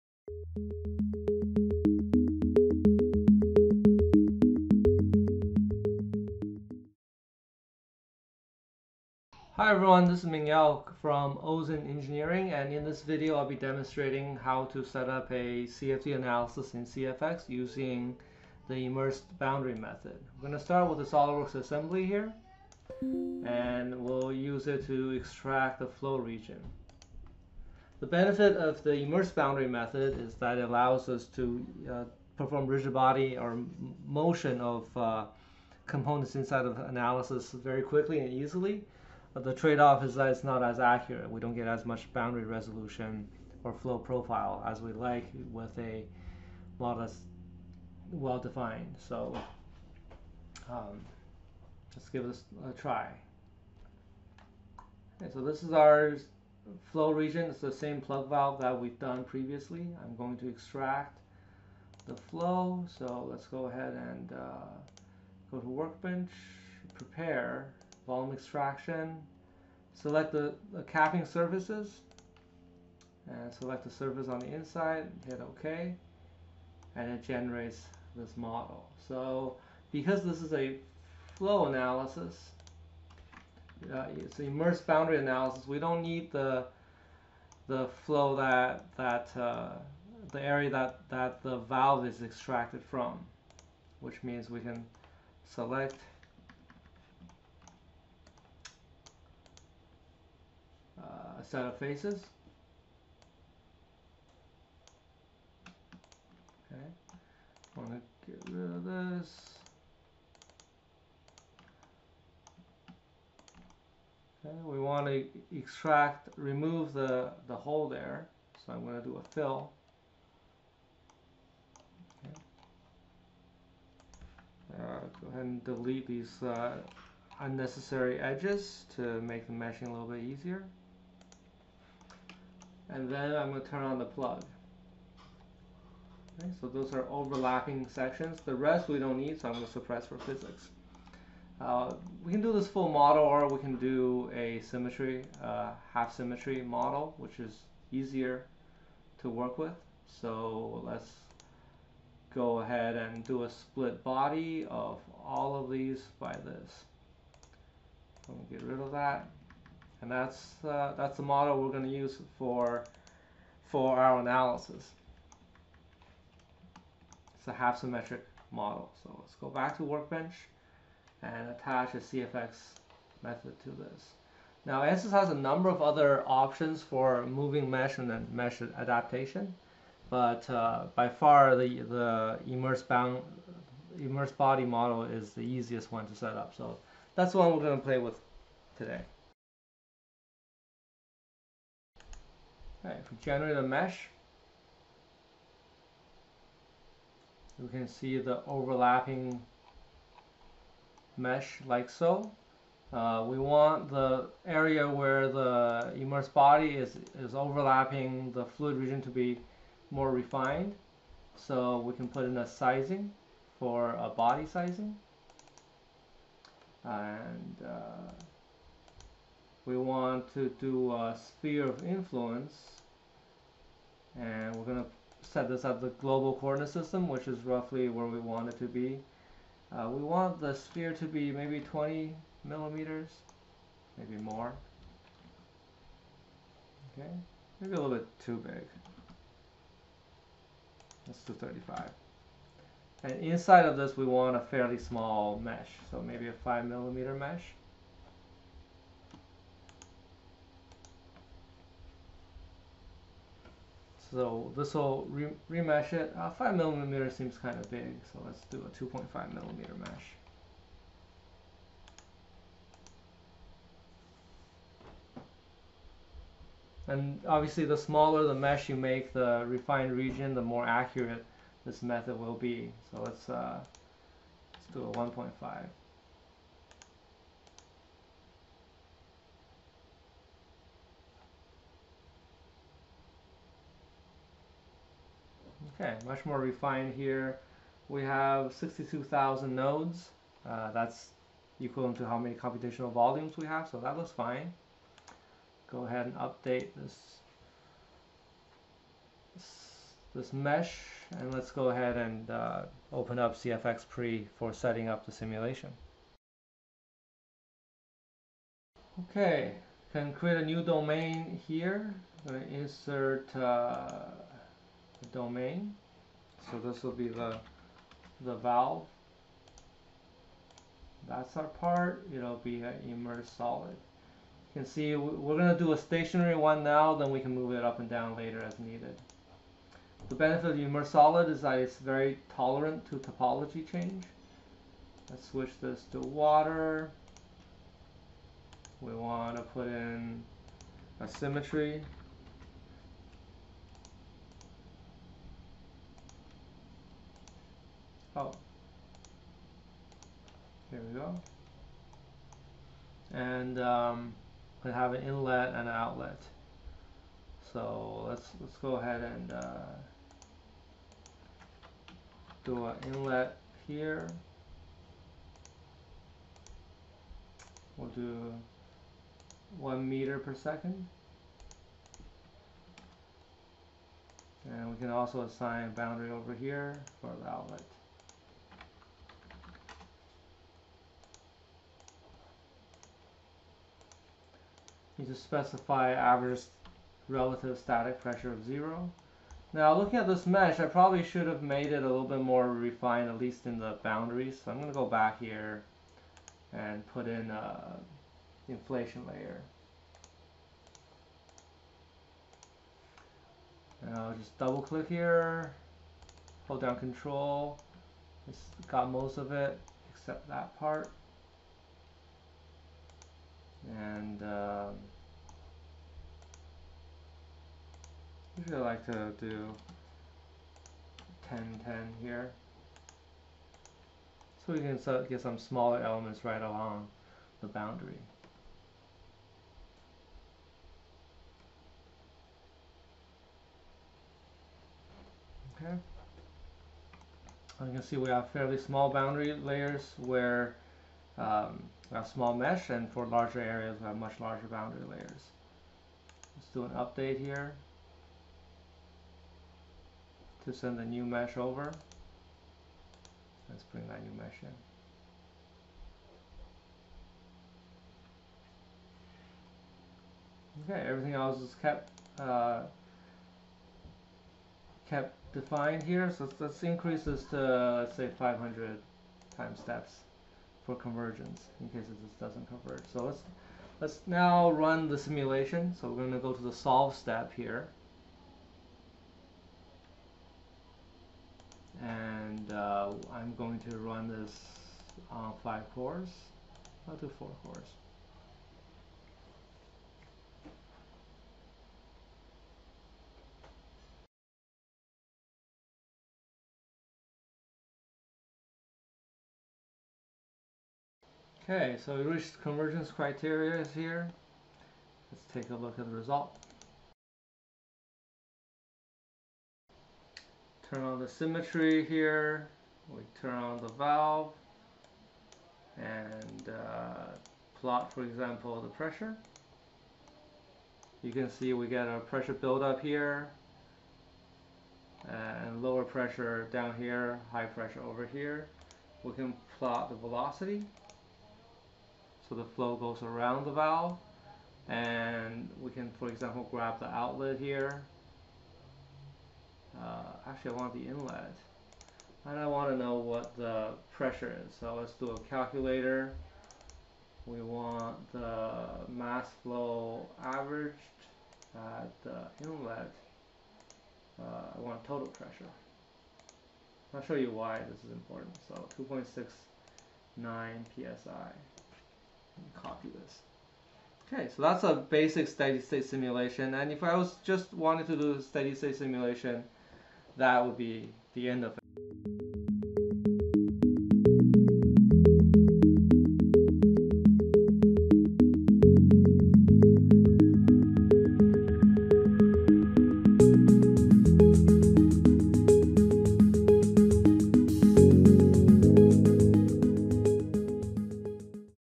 Immersed body valve simulation using Ansys CFD: Part1
Valve simulation using immersed body in Ansys CFD. Immersed body is a very fast and simple way to model move parts in CFD. It is very robust and reliable with some trade off in accuracy. In this part 1 video we do a basic steady state simulation of a plug valve.